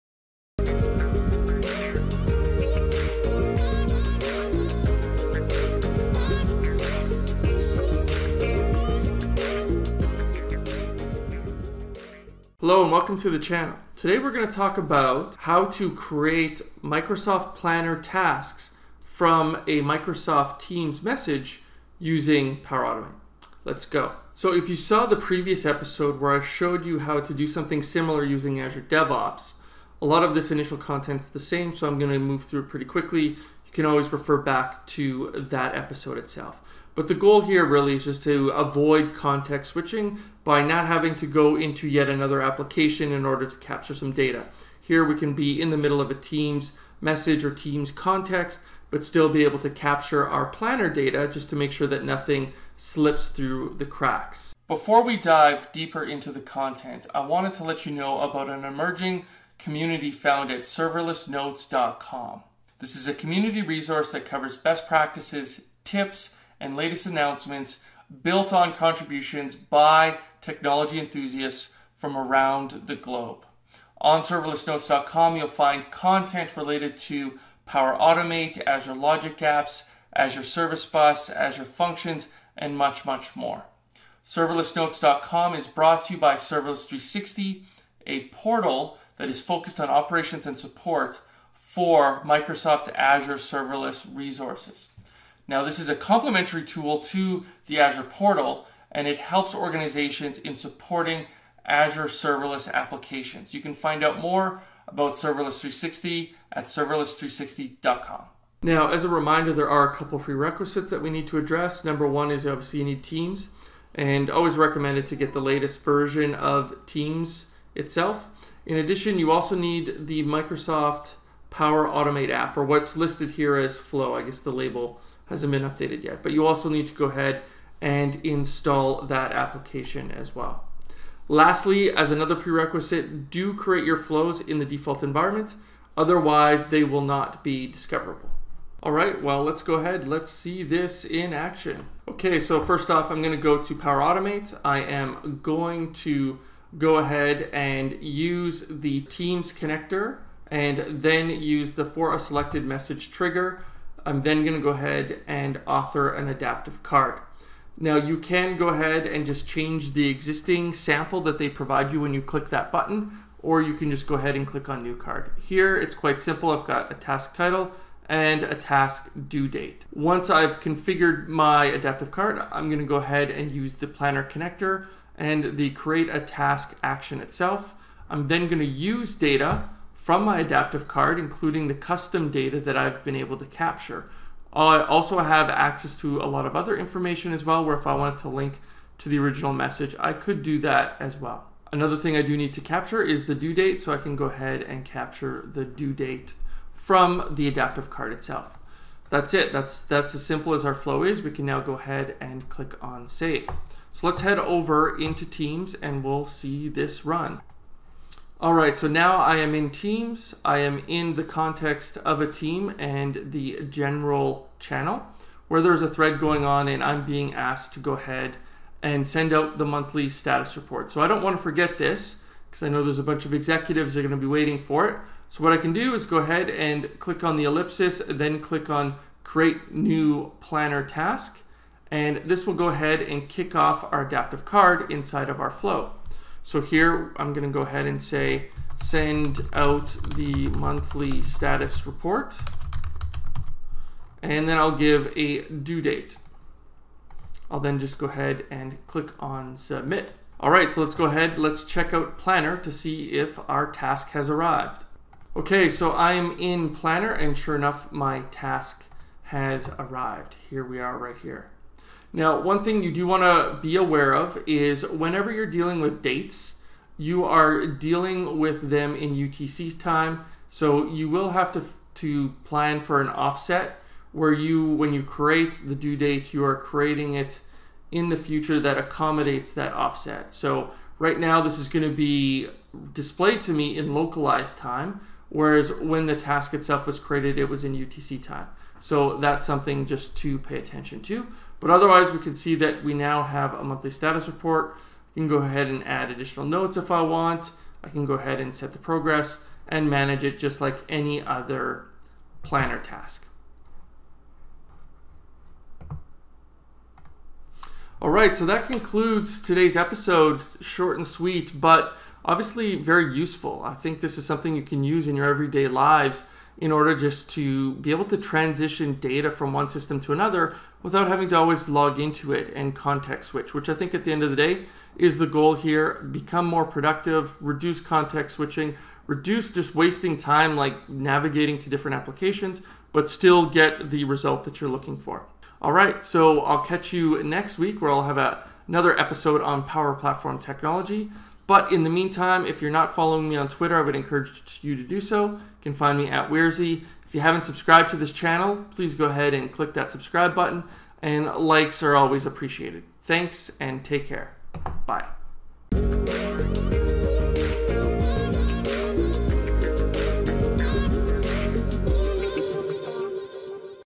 041 - Creating Planner Task from Microsoft Teams Chat Message
In this video we are going to discuss a new feature found in Microsoft Teams/Power Automate that allows a user to kick off a flow from a posted message. The scenario we are going to dive deeper into involves using information from our conversation to create a new Task in Microsoft Planner. We will also be able to collect more information from an Adaptive Card which will be shown to our user.